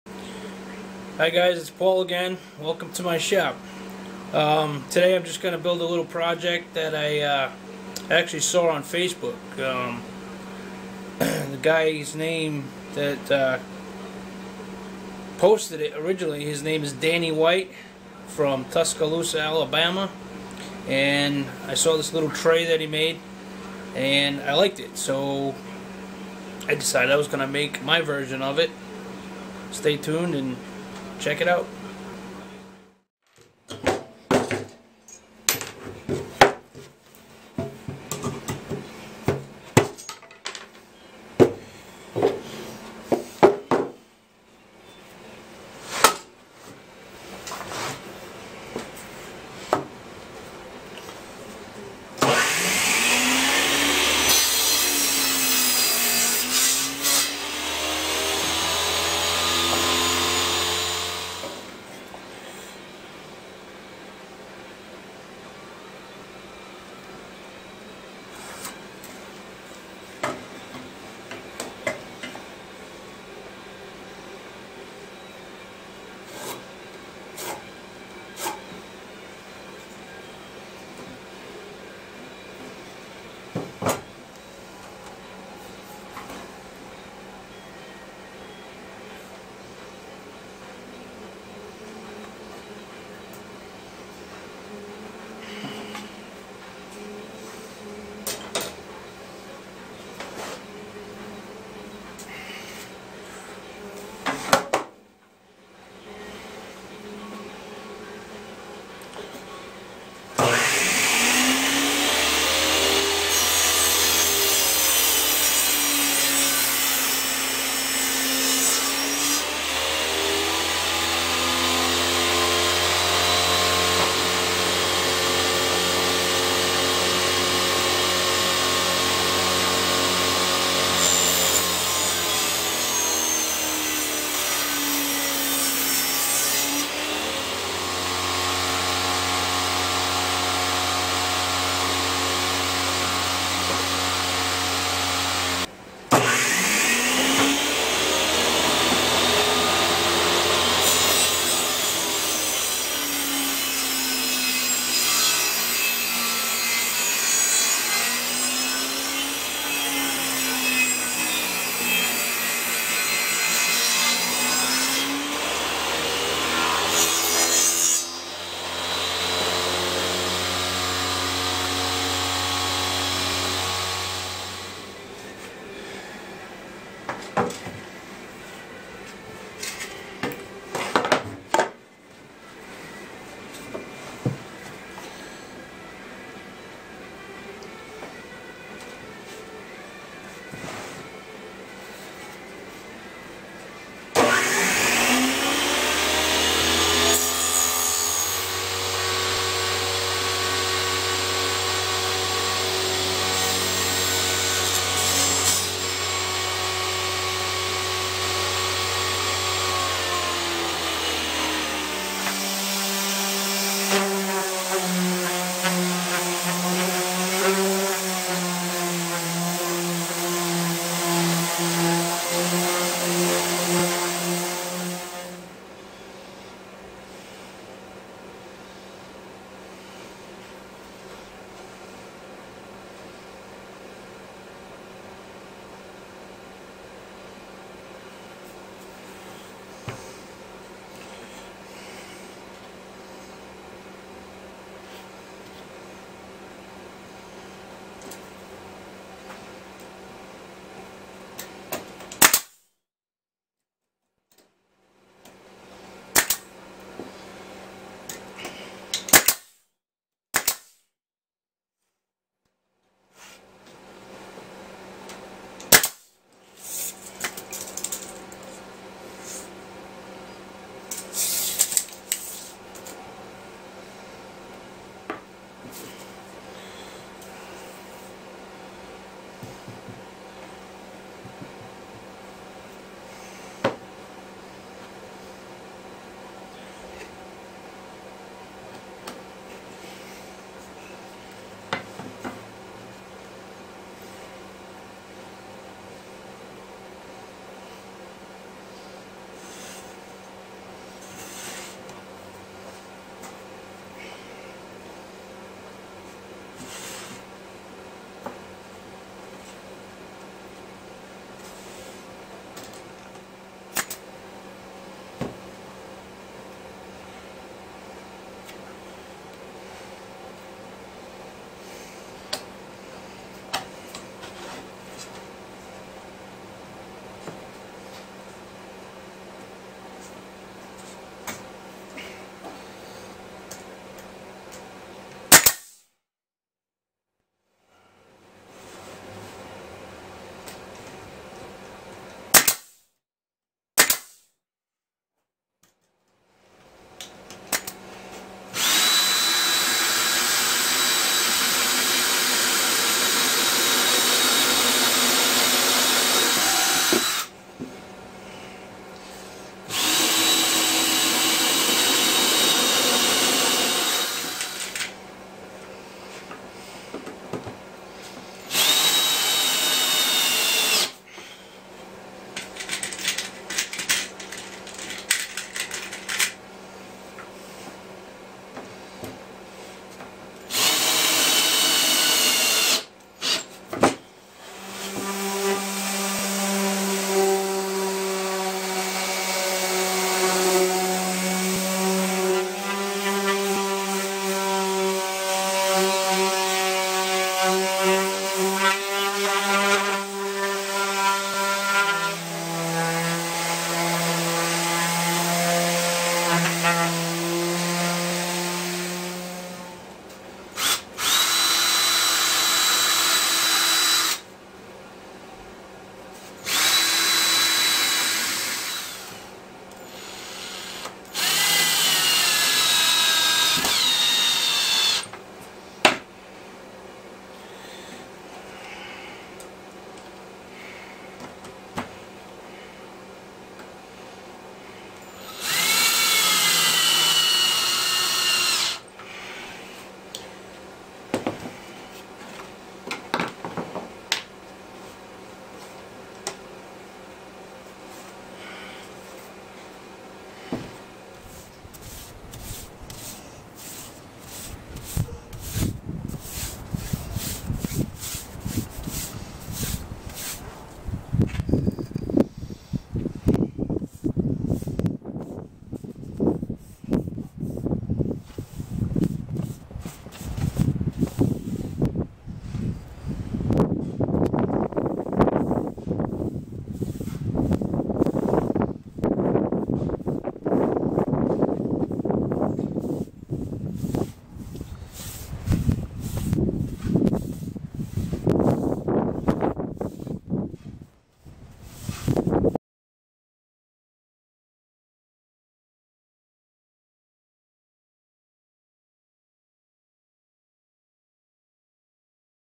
Pallet wood serving tray PMW #04
In this video, I show you how I made a serving tray, out of pallet wood.
 I got the idea from a Facebook post, posted by Danny White, from Tuscaloosa, Alabama. I liked his idea, made a few changes, and created my own version.
As always, if you like what you see, please give me a "thumbs up", and share if you like.

My Links